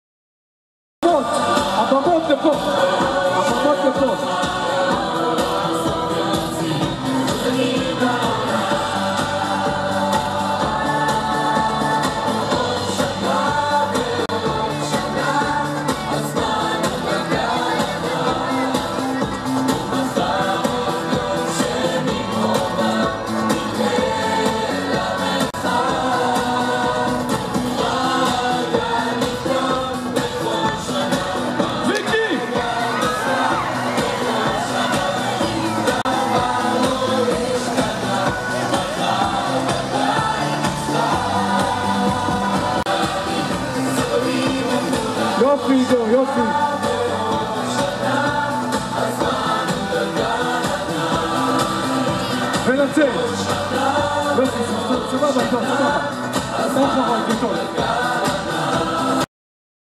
Keren Bergman Gymnastic Wheels 04-20-2015
Keren Bergman - Zichron Yaakov Gymnastic Wheels,April 20th,2015
Preparing for Israel 67's Independence Day - Jerusalem